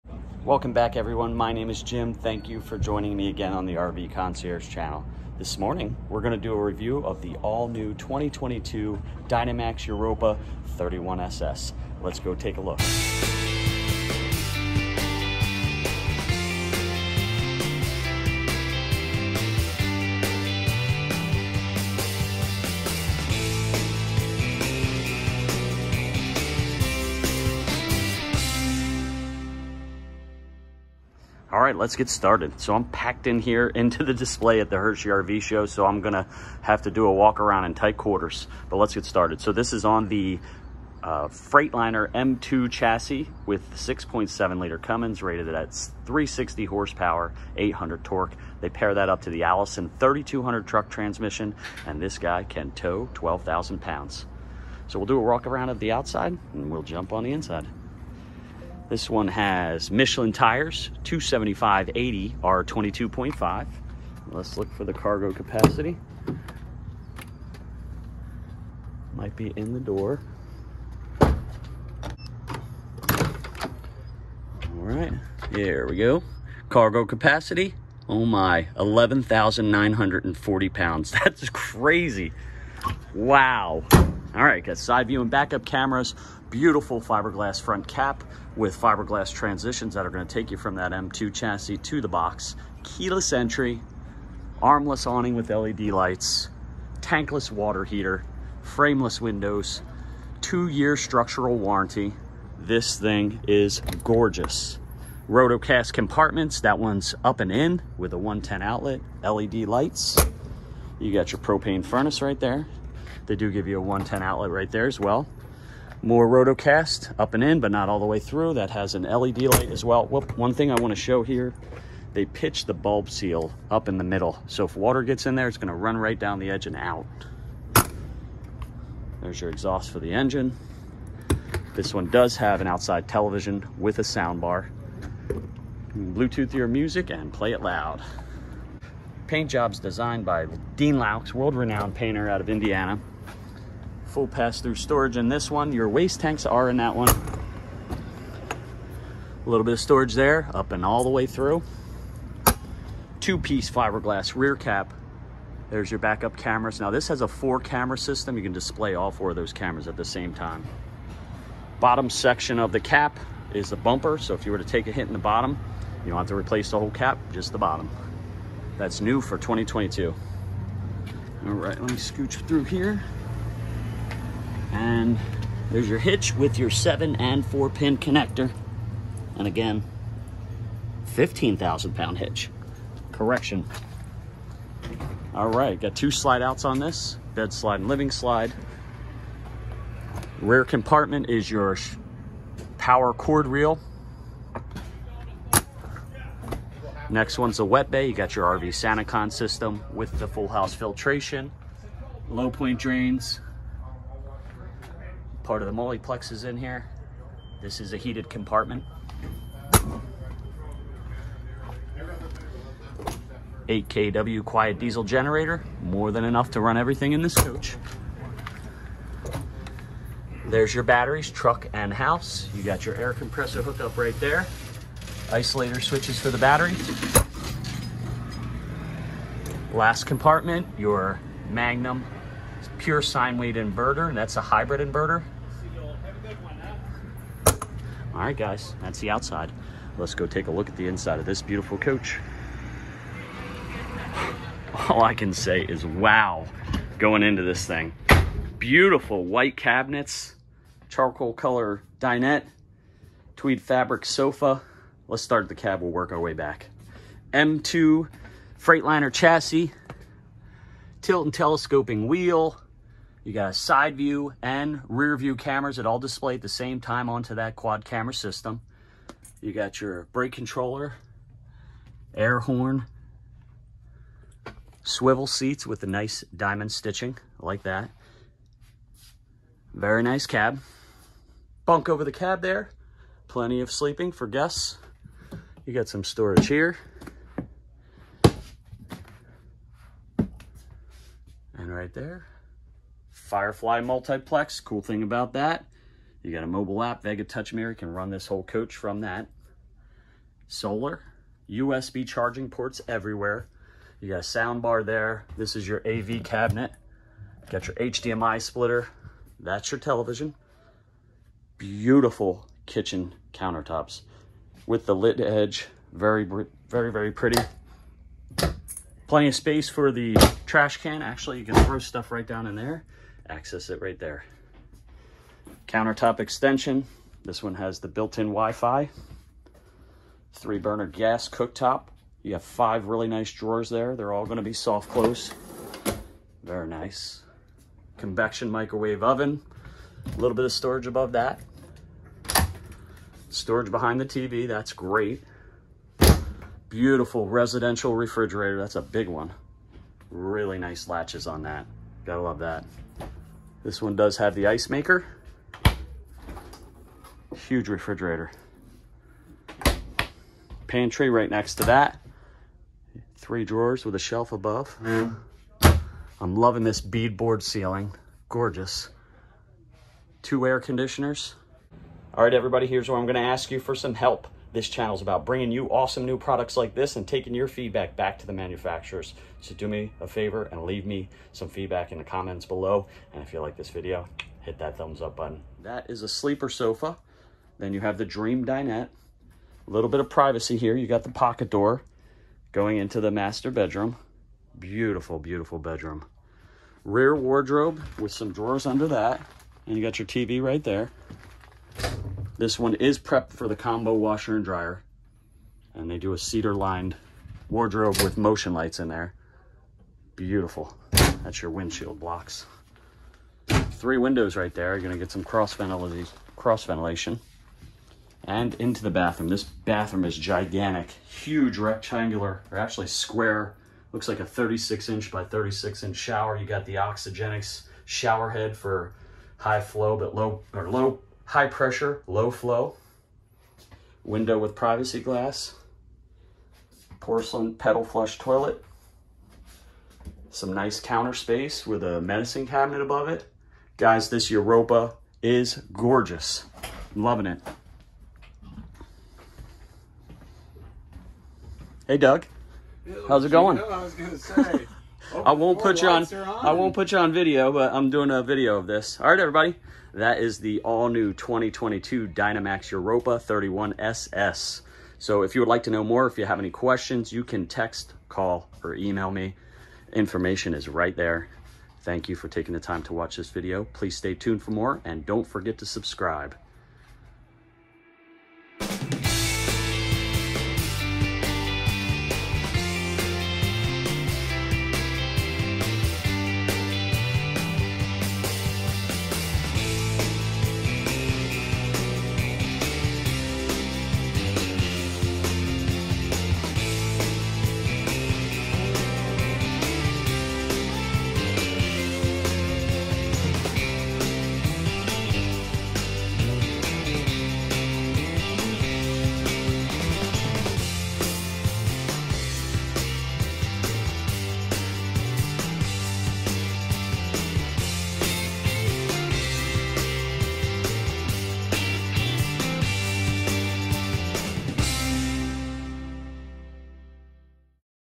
2022 Dynamax Europa 31SS
This all new re-imagined Dynamax Europa is gorgeous! This rig features a beautiful new white cabinet interior with white bead board ceiling, on demand hot water, tons of power and with huge towing and cargo carrying capacities!